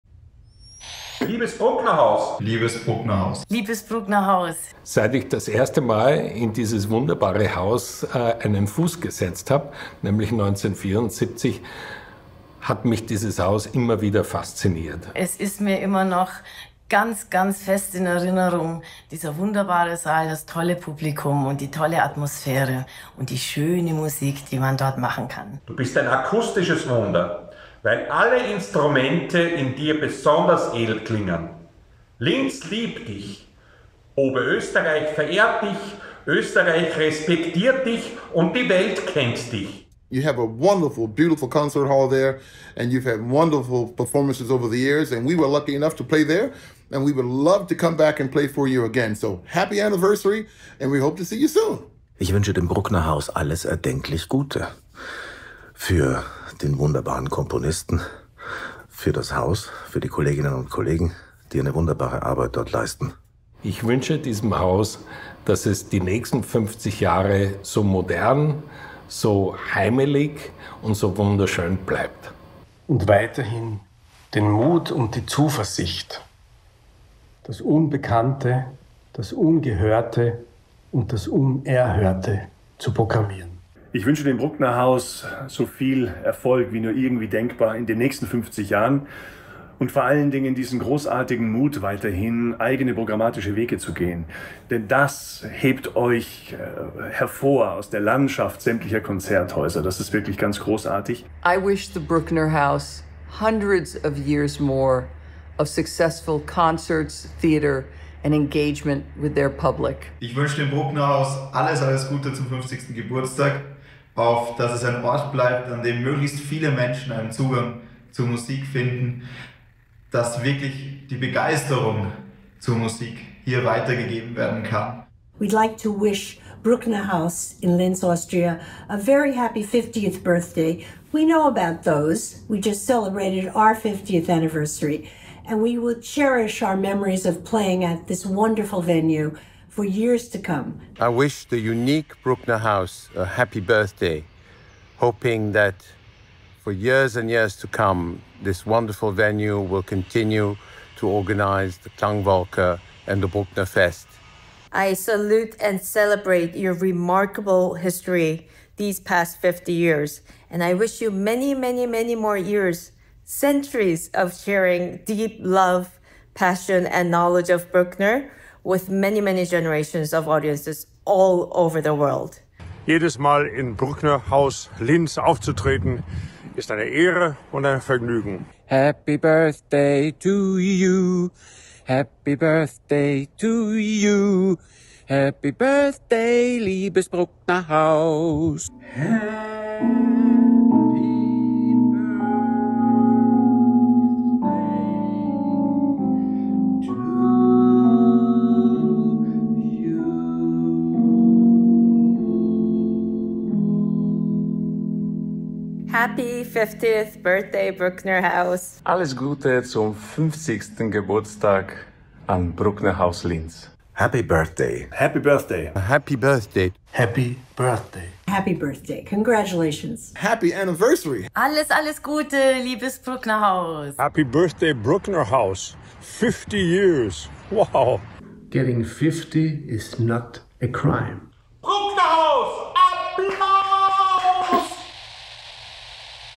Happy Birthday, Brucknerhaus Linz! Grußbotschaften
Die ganze Welt feiert das Jubiläum des Brucknerhauses Linz: Internationale Künstler*innen haben ihre ganz persönlichen Grußbotschaften und berührenden Glückwünsche an das Brucknerhaus Linz gesendet: Es sind liebevolle Erinnerungen und Herzenswünsche, die von Persönlichkeiten wie Franz Welser-Möst, Dirigent des Cleveland Orchestra, Perkussionist Christoph Sietzen, Schauspiel-Star Cornelius Obonya, Chefdirigent des Bruckner Orchester Linz Markus Poschner, Mitgliedern des berühmten Count Basie Orchestra, den Sänger*innen von The Manhattan Transfer, Regisseurin Francesca Zambello, dem österreichischen Trompeter Thomas Gansch oder Dirigentin Han-Na Chang gesendet wurden.
Konzeption und Idee: grauwerk.